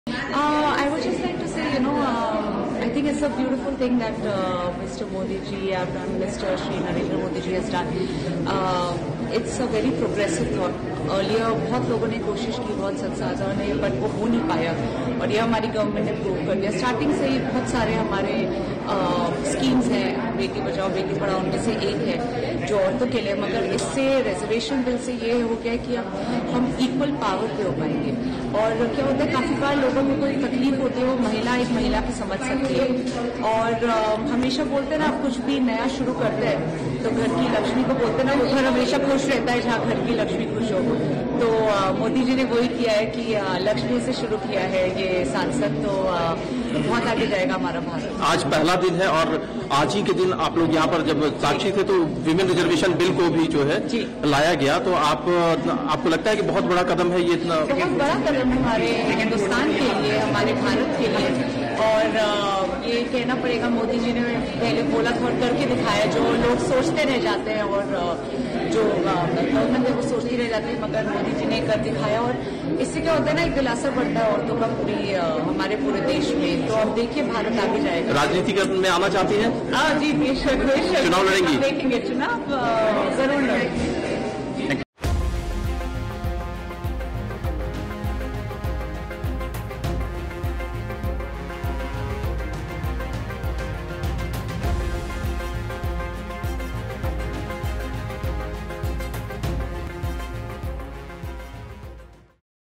প্ৰধানমন্ত্ৰী মোদীক ধন্যবাদ অভিনেত্ৰী ঈষা গুপ্তাৰ
▶️বহুত আগলৈ যাব আমাৰ ভাৰত। ৩৩% আসন সংৰক্ষণে সম অধিকাৰ প্ৰদান কৰিব মহিলাসকলক। প্ৰধানমন্ত্ৰী মোদীক ধন্যবাদ অভিনেত্ৰী ঈষা গুপ্তাৰ।